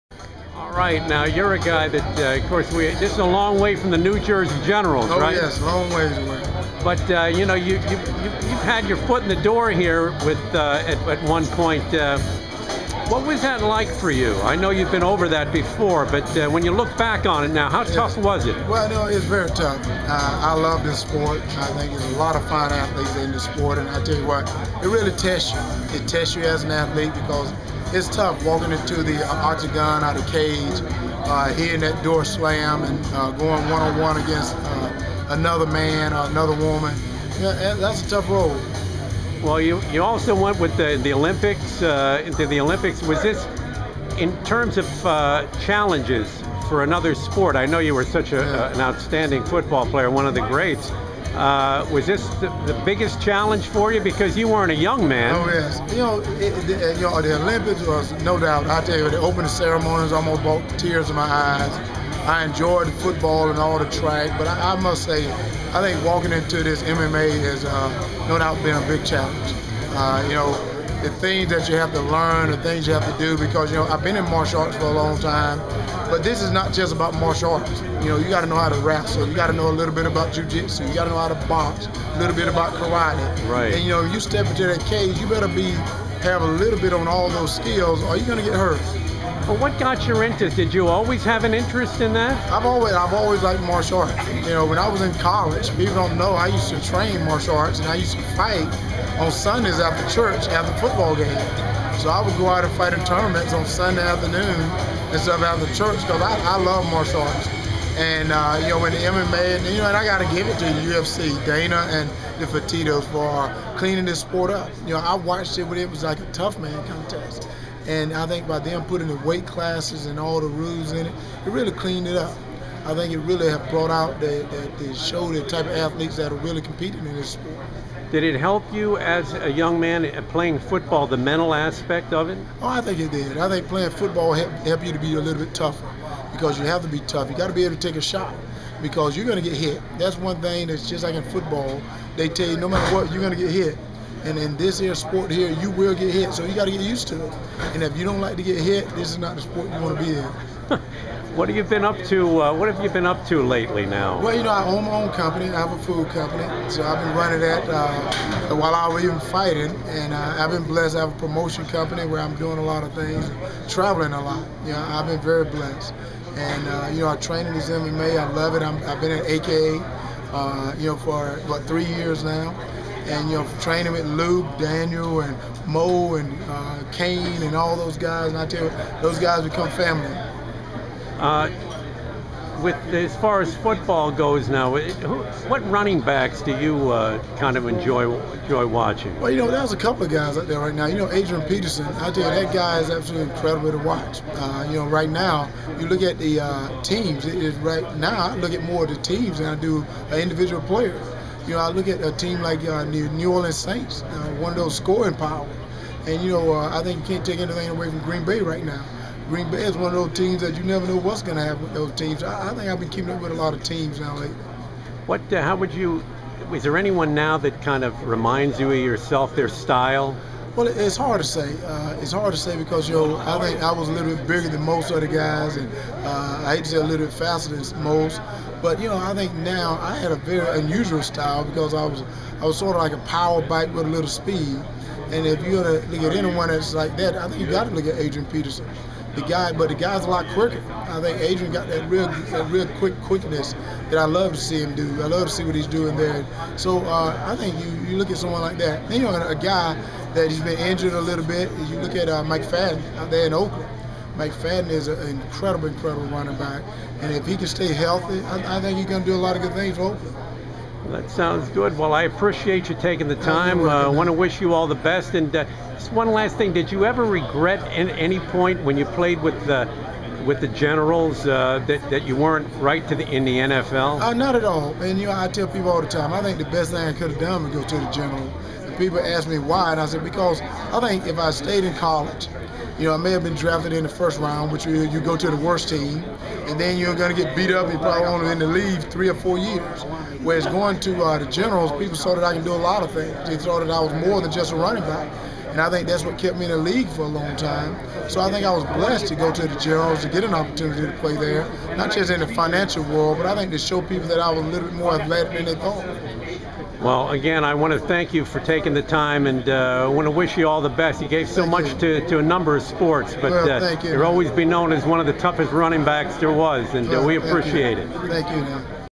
Herschel Walker "Three Sport Man"
The former USFL and NFL Star running back speaks about his career with the New Jersey Generals, his experience as a member of the U.S Olympic bobsled team and his competing in the Strikeforce MMA organization.